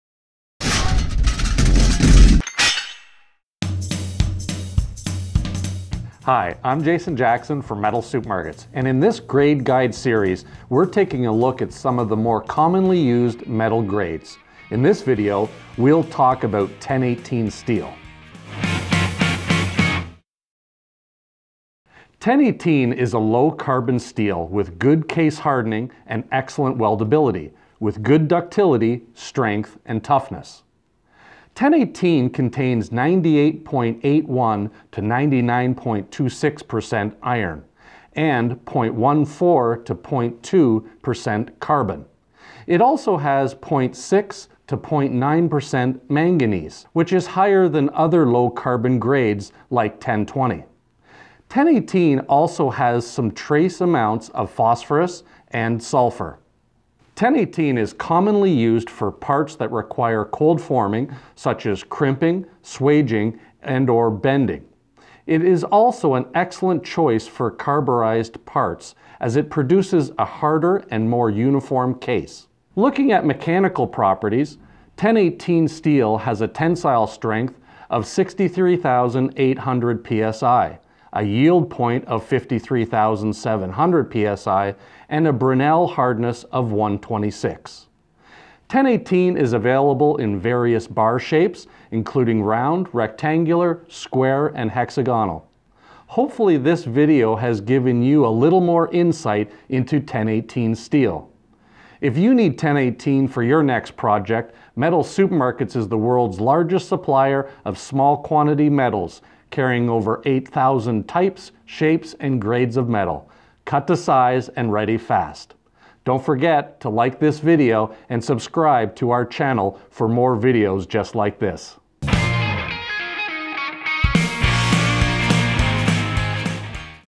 1018 Steel Grade Guide | Metal Supermarkets
In this Grade Guide video, we take a closer look at the chemical and mechanical properties of 1018 Steel.